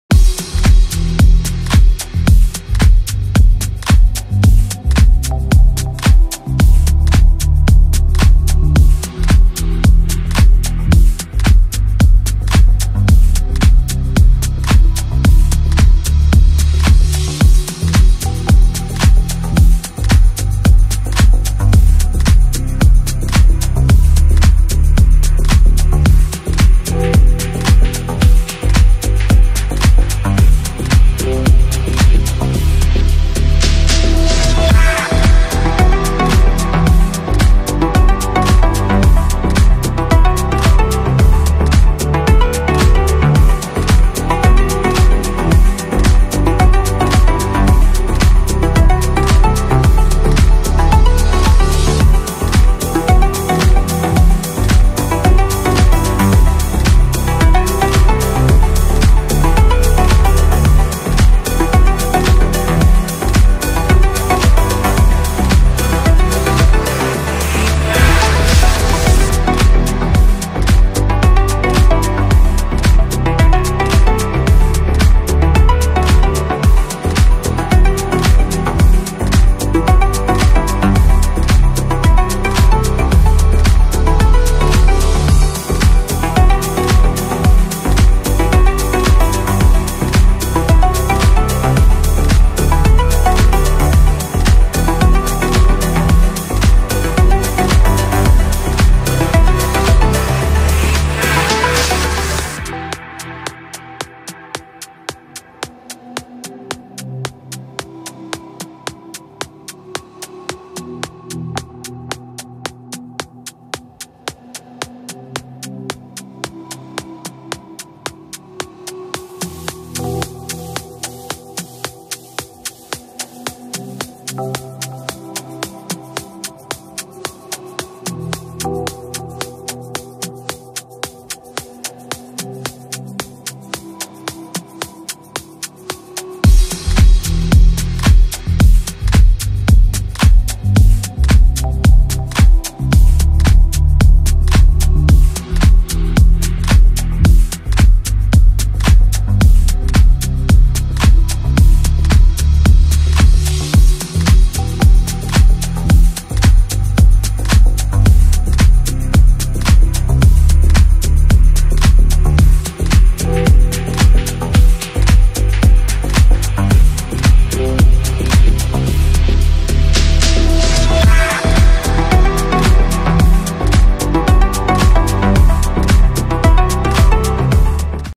2 день 3000 м, девушки, финал
Харьков, 15 июня 2017 г. Чемпионат Украины по легкой атлетике среди юношей и девушек 2002 и младше. Фото и видео